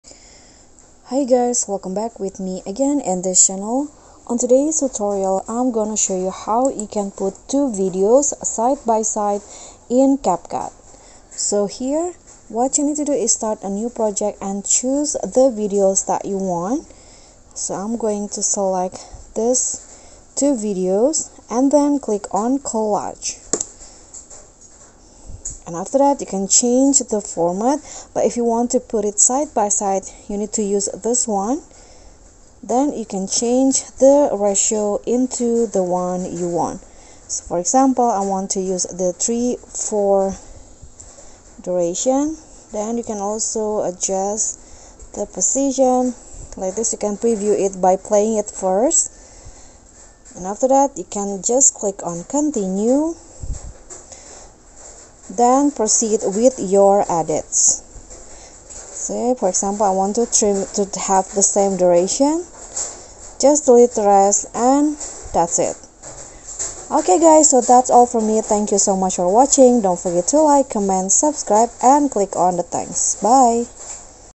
How to Put Two Videos Side by Side CapCut| How Can You Combine Two Videos in CapCut? NEW UPDATE 2023
Here's how you can put two videos side by side in CapCut